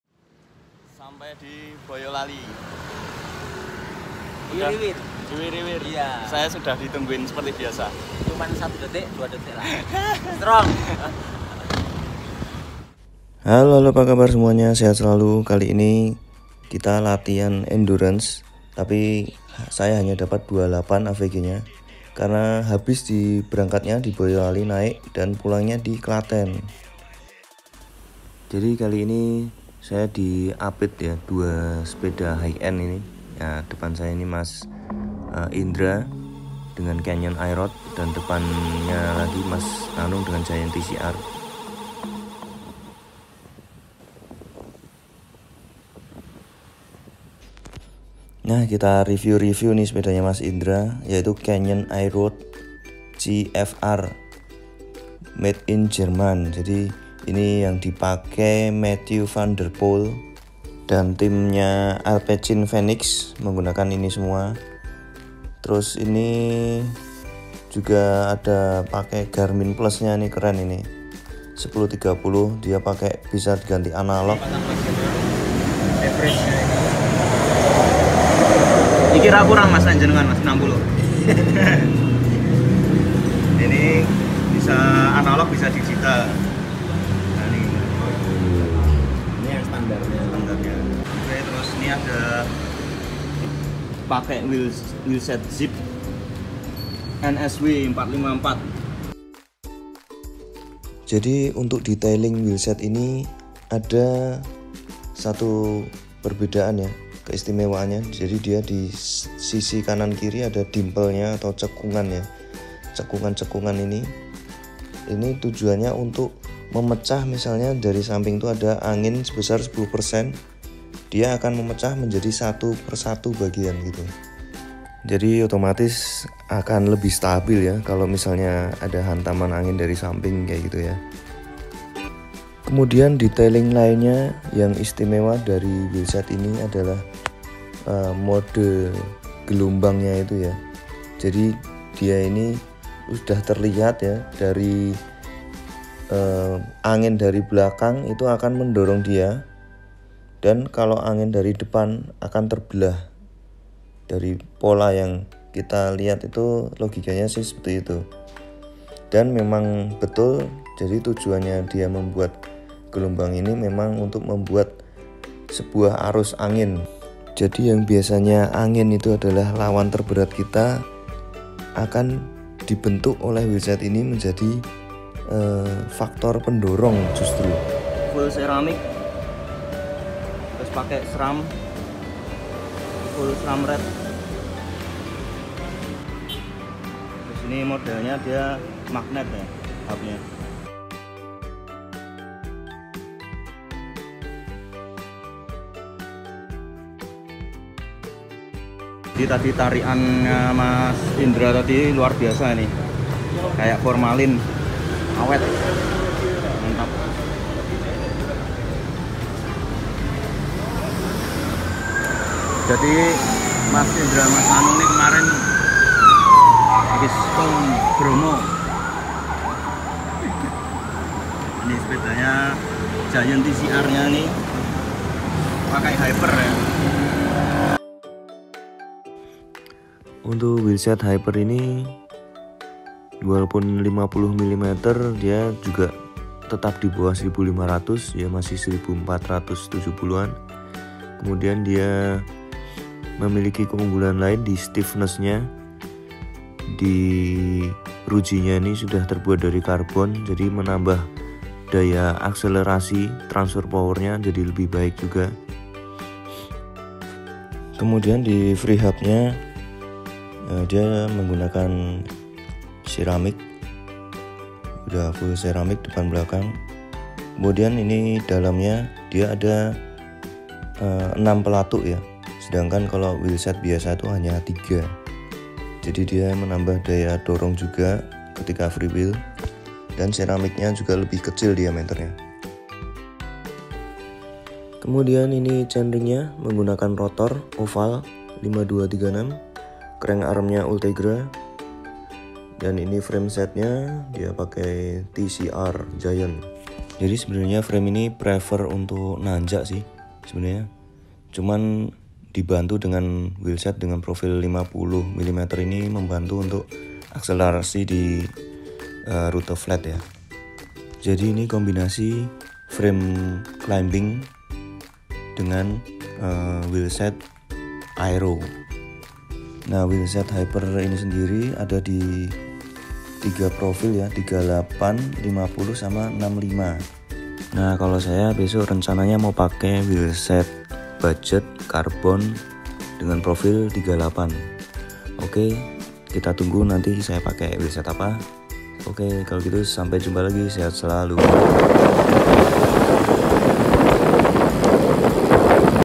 Bike check sepeda High end wawasan harga dan kualitas
Endurance 150 KM untuk pe hobies sudah top berada di avg 31-33 kmh , untuk saya yang baru mengawali lagi gowes terhenti selama 3 bulan cukup berada di avg 28
Terbantu ban GP 5000 yg paling tidak membantu rolling resistance , berhasil nempel di rute flat